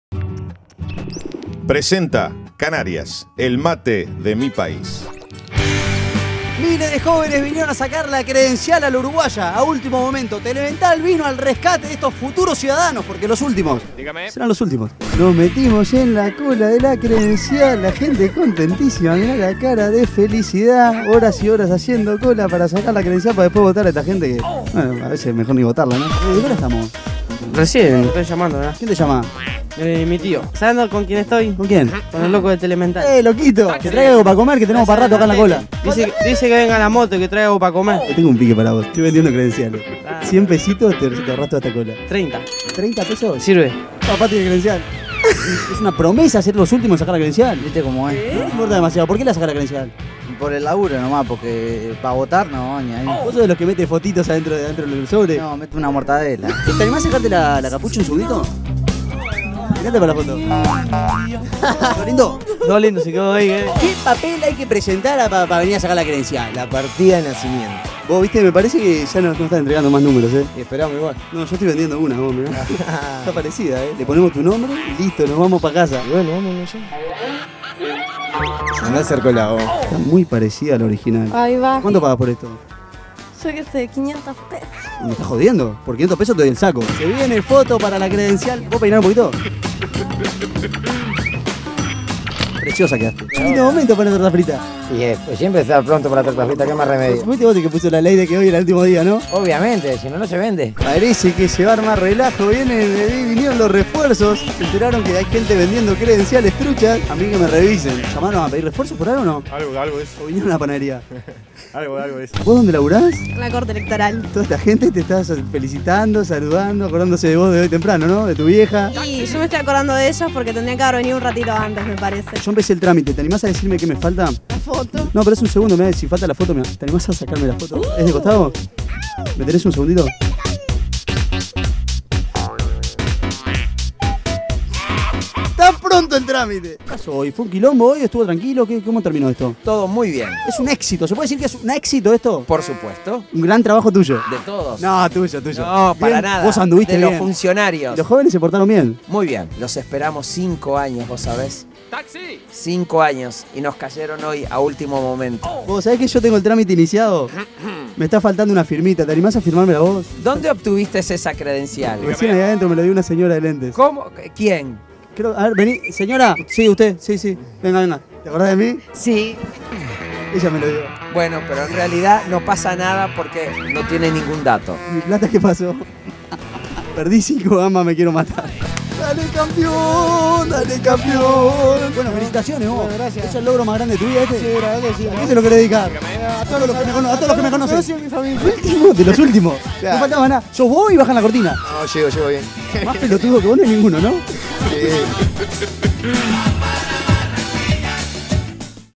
CREDENCIAL a ultimo momento
miles de uruguayos deicden savar la credencial el ultimo dia .